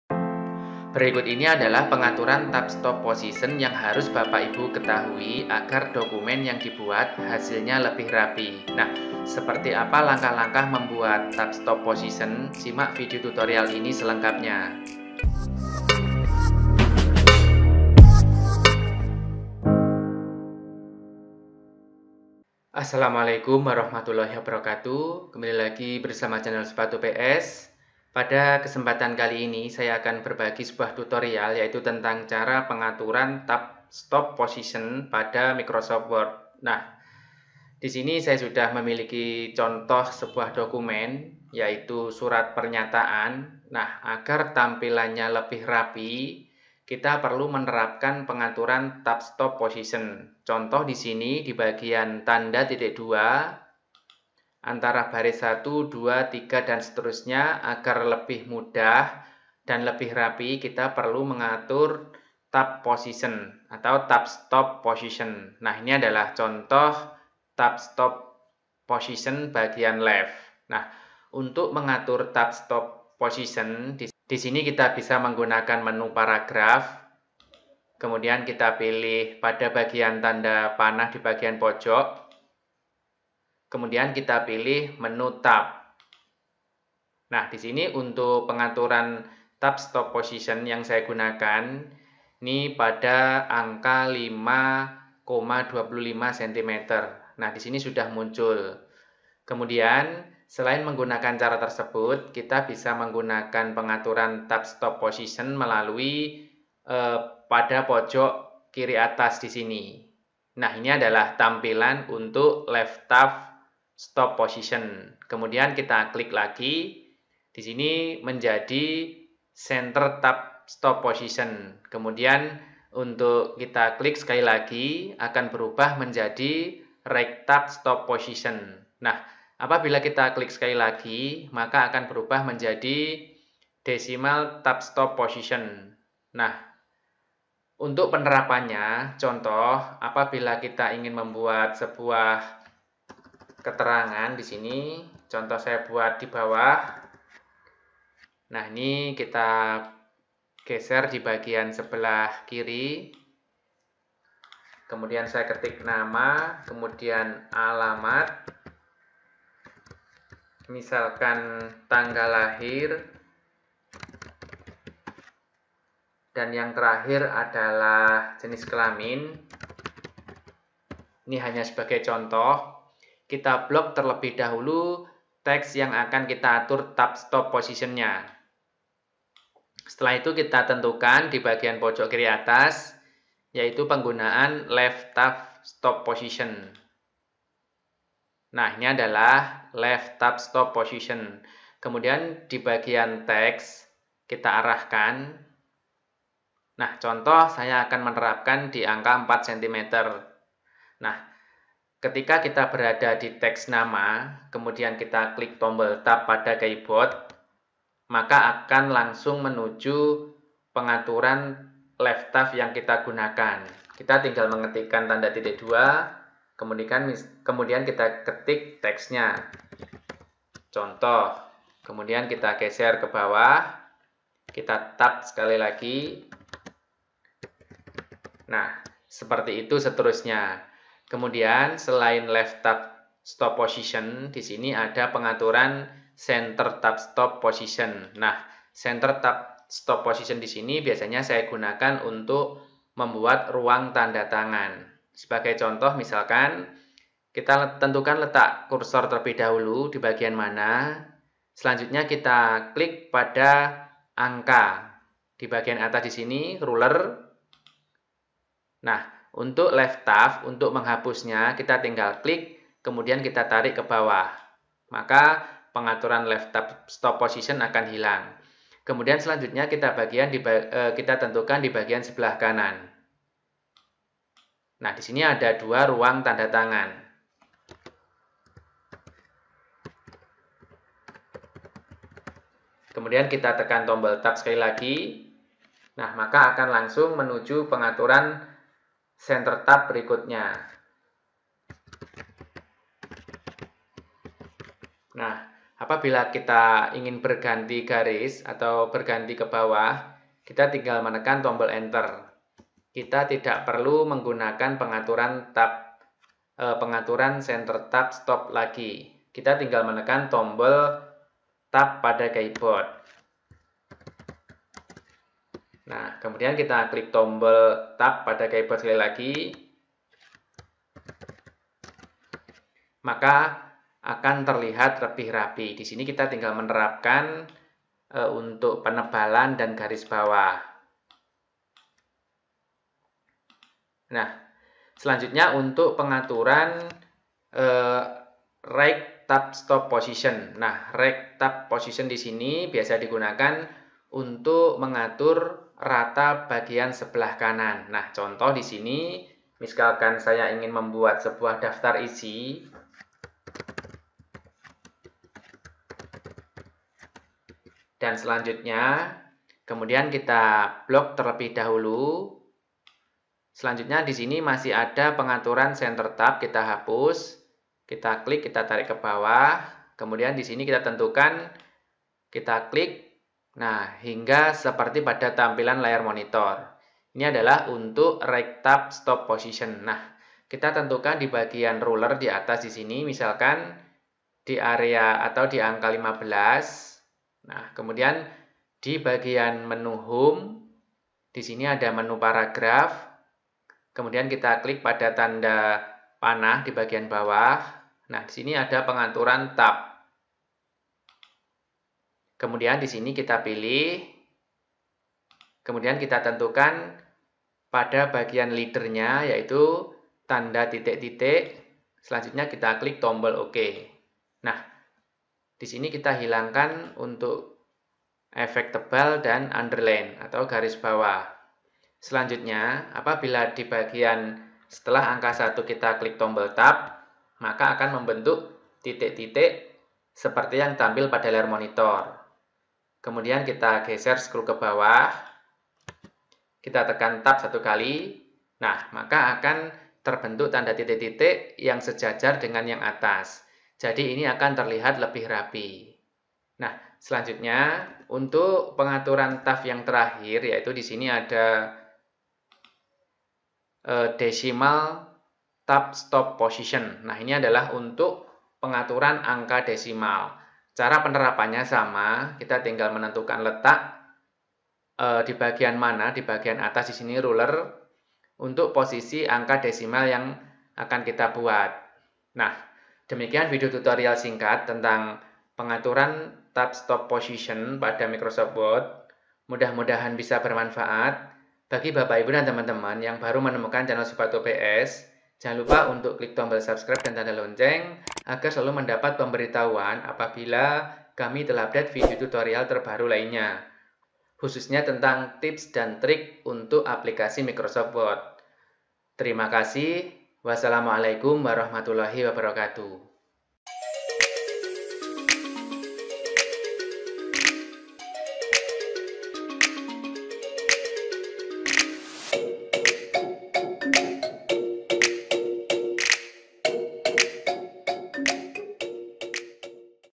Cara Membuat dan Mengatur Tab Stop Position di Microsoft Word
Video tutorial tentang cara membuat dan mengatur tab stop position. Adalah cara mengatur tabulasi otomatis. Agar hasil dokumen yang dibuat lebih rapi.

Langkah-langkah atau cara membuat dan mengatur tab stop position adalah sebagai berikut :
1. Pilih menu home.
2. Pilih group paragraph.
3. Klik tanda panah di pojok bawah.
4. Klik tombol tab.
5. Atur letak dan aligment.
6. Klik tombol OK.

Selain dengan cara diatas kita dapat menggunakan cara membuat dan mengatur tab stop position melalui edit tab dibagian pojok kiri atas. Ada menu tab yang bisa kita gunakan agar tampilan teks lebih rapi pada dokumen word.
Left Tab Stop Position fungsinya untuk rata tab stop kiri.
Center Tab Stop Position fungsinya untuk rata tab stop tengah.
RightTab Stop Position fungsinya untuk rata tab stop kanan.
Decimal Tab Stop Position fungsinya untuk rata tab stop angka desimal.

Untuk membuat ruang tanda tangan atau tempat tanda tangan biasanya digunakan center tab stop position, sedangkan untuk  daftar isi biasanya digunakan right tab stop position.

Semoga video tutorial tentang cara membuat dan mengatur tab stop position dapat bermanfaat.